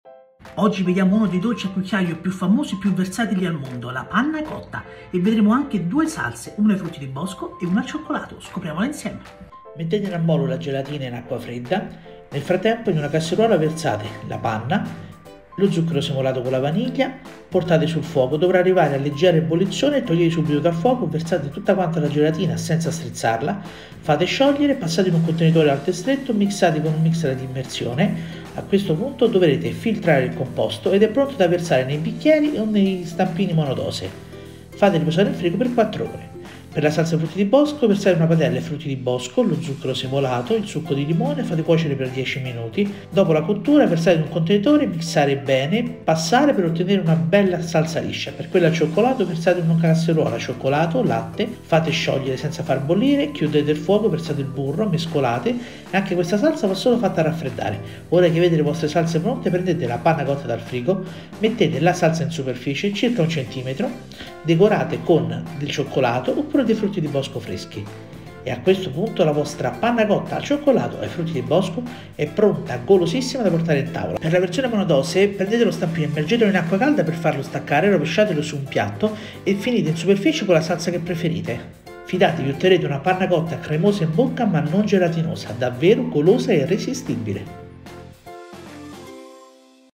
Le ricette in 90 secondi !! LA PANNA COTTA!!
Oggi ricettina extra per il dolce al cucchiaio più amato e versatile, parlo delle ✨PANNA COTTA✨, golosa ne cremosa perfetta da accompagnare a qualsiasi salsa.

SALVATE IL VIDEO📹✅ per averlo a disposizione, sono sicuro che sarà uno dei vostri dessert fine pasto preferiti.

Per prepararla basta seguire pochissimi consigli:
🔹Per questa ricetta dovrete usare la classica panna fresca, quella del banco frigo non zuccherata
🔹Potete sostituire la colla di pesce con 2 gr agar agar che però dovrà bollire per 4 minuti nella panna, però otterrete una consistenza totalmente diversa.
🔹Mixare il composto aiuterà ad emulsionarlo e filtratelo per evitare grumi della colla di pesce o della vaniglia.
🔹Potete usare i frutti di bosco che preferite oppire fare un mix come ho fatto io, l’importante è cuocerli poco e filtrare per togliere i semini se volete una salsa liscia.
🔹Fate riposare in frigo 4 ore ma anche di più la panna cotta, può essere preparata in anticipo.
Aspettate che le salse siano fredde prima di usarle
🔹La panna cotta si conserva in frigo ben coperta con la pellicola per 3 giorni.

Trovate la ricetta completa anche sul mio blog
💻PASTIGIOCA.IT cliccando sul